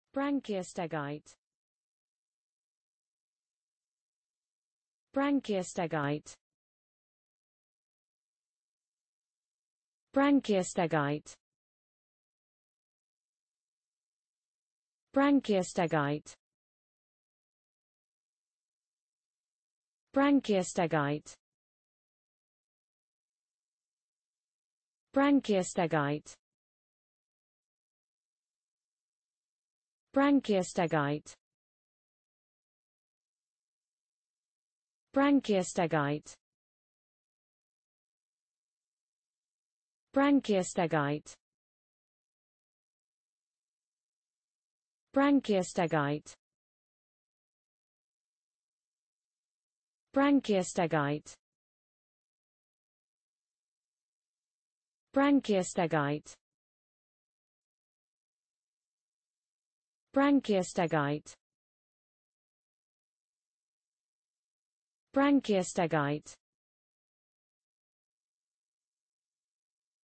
Branchiostegite: How to pronounce Branchiostegite in english (correct!).Start with B. Learn from m
Follow me!Learn from me how to pronounce the word Branchiostegite in english.This video shows you how to pronounce the word Branchiostegite in english.

null